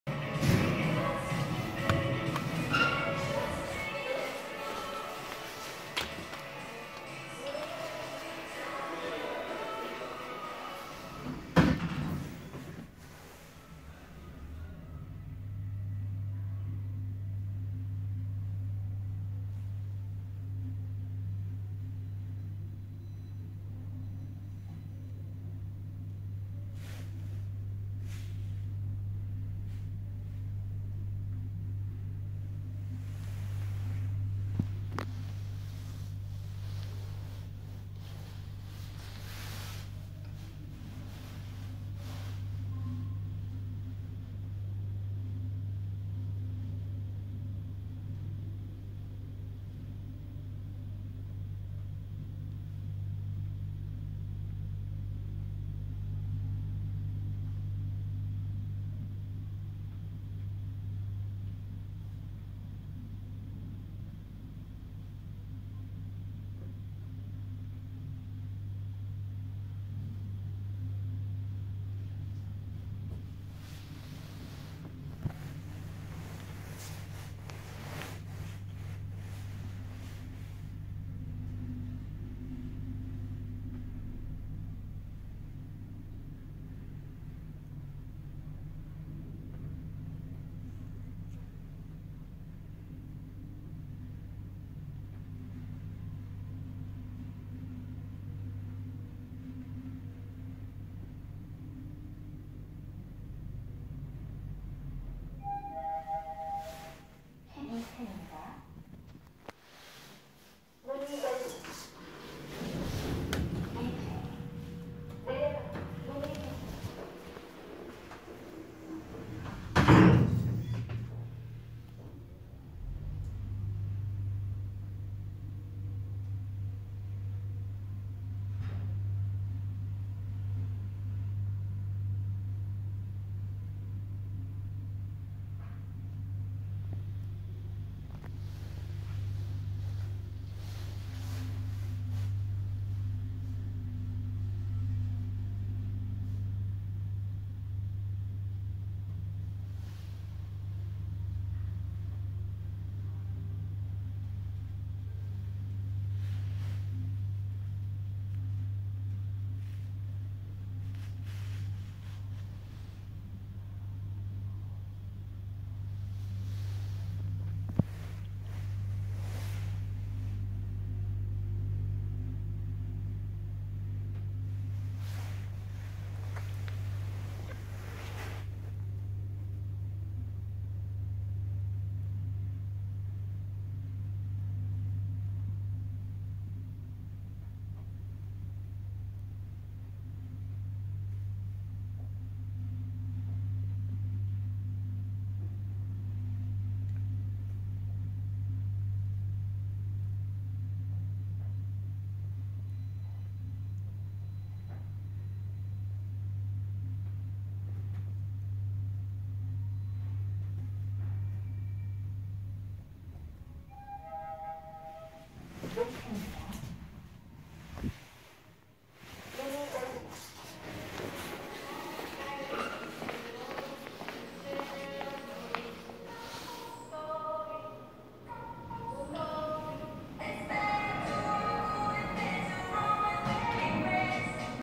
서울특별시 송파구 올림픽로 240 (잠실동) 롯데월드몰 나머지구간 18 현대엘리베이터 탑사기
이 엘리베이터는 로프식-VVVF 45m/min라서 매우 느리며, 이곳은 직원들이 타는곳이니 탑사시 주의하시기 바랍니다.